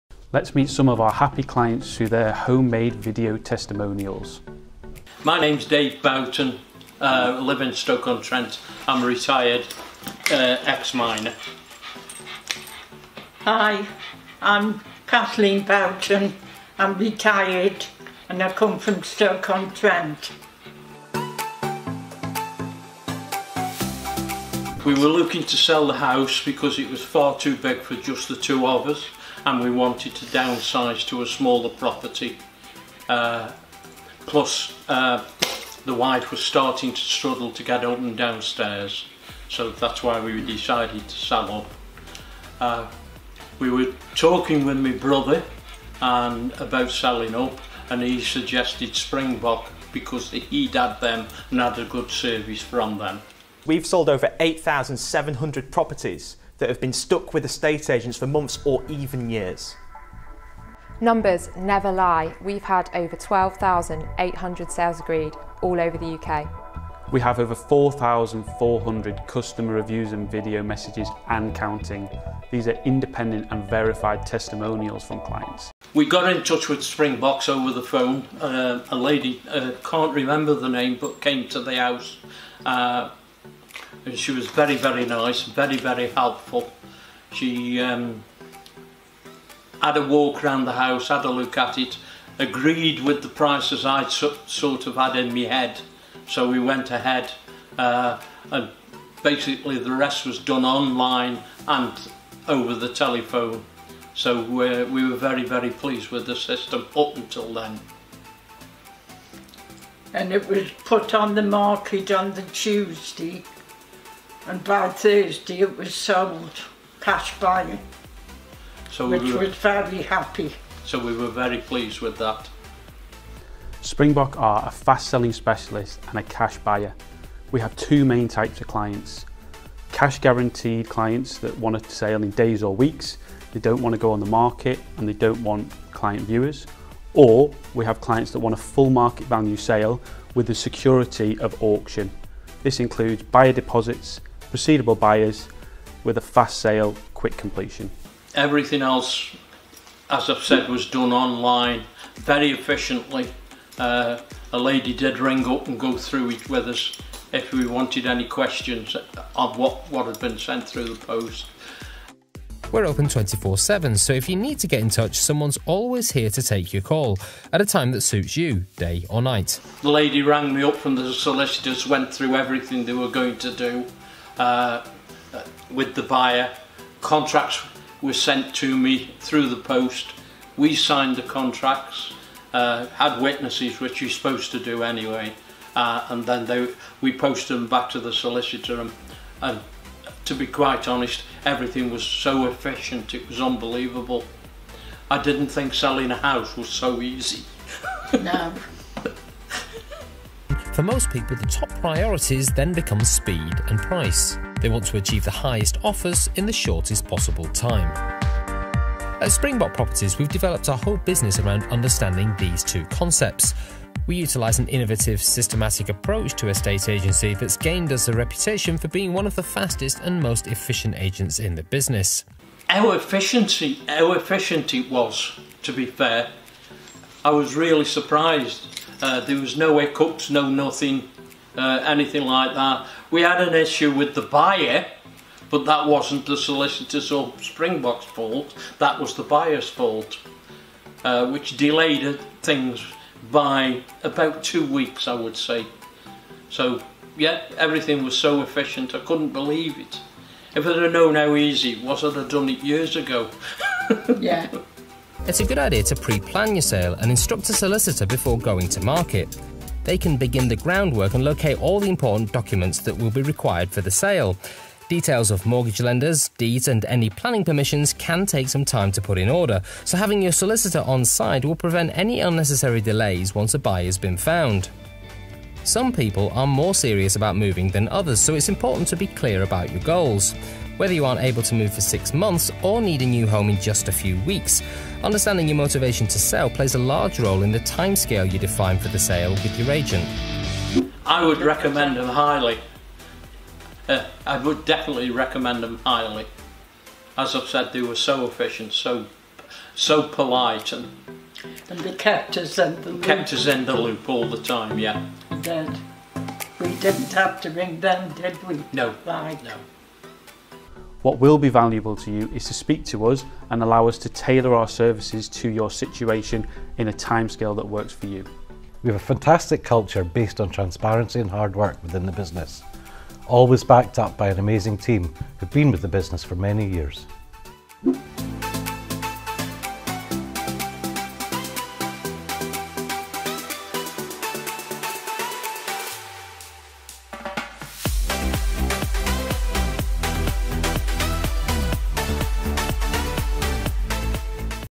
Springbok Properties helped retired couple David and Kathleen sell their property - Review series
David and Kathleen wanted to downsize to a smaller property and were advised by a friend to contact Springbok Properties for a convenient and hassle free sale. Springbok found them a cash buyer for the price they requested.